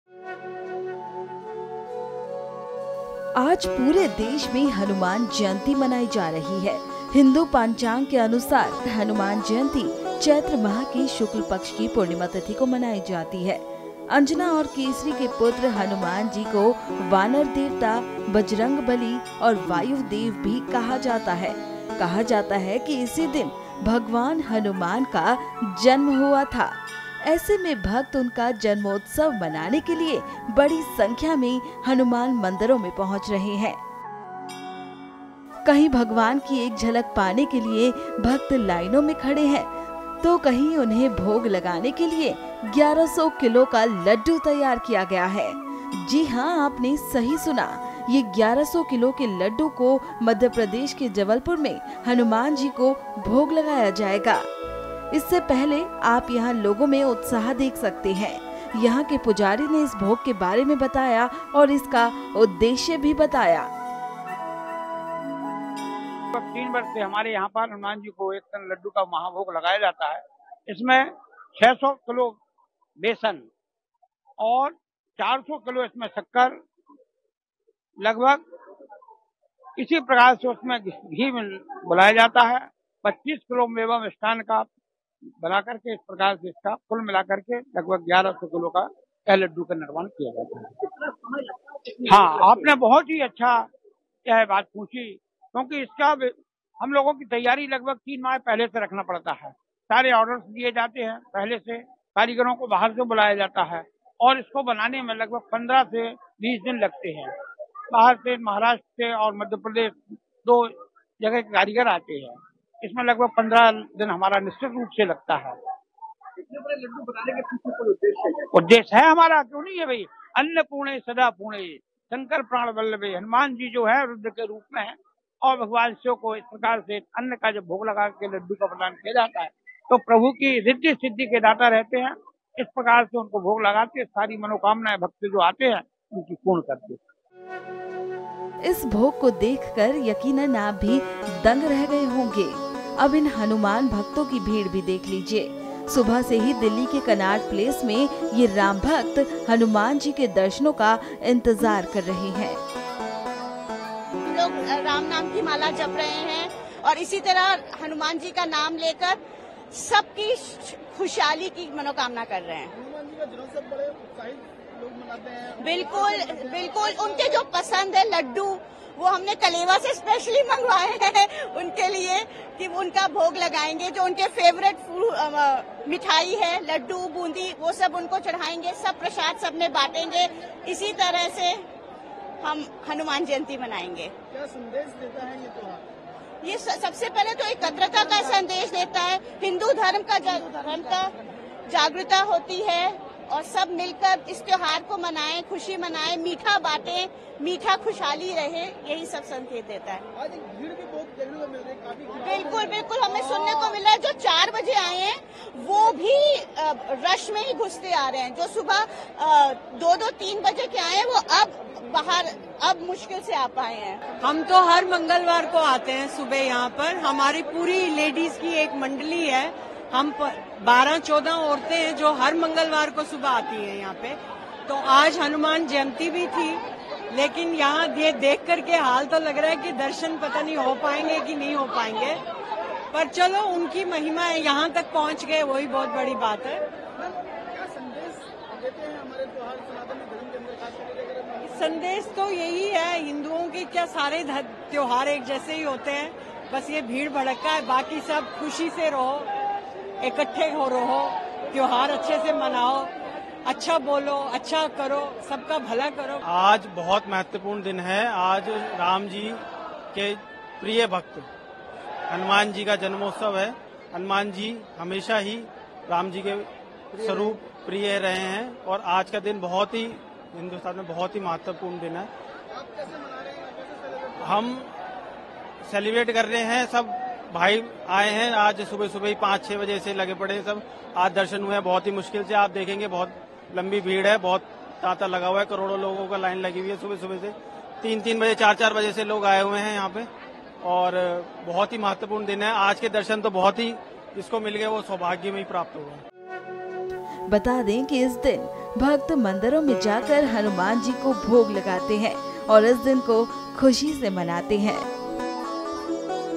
Hanuman Jayanti Celebration: देशभर में Hanuman Jayanti की धूम, 1100 किलो के लड्डू का लगेगा भोग
Hanuman Jayanti 2024: आज पूरे देश में हनुमान जयंती मनाई जा रही है। हिंदू पंचांग के अनुसार, हनुमान जयंती चैत्र माह के शुक्ल पक्ष की पूर्णिमा तिथि को मनाया जाती है. अंजना और केसरी के पुत्र हनुमान जी को वानर देवता, बजरंगबली और वायु देव भी कहा जाता है. कहा जाता है इसी दिन भगवान हनुमान का जन्म हुआ था. ऐसे में भक्त उनका जन्मोत्सव मनाने के लिए बड़ी संख्या में हनुमान मंदिरों में पहुंच रहे हैं।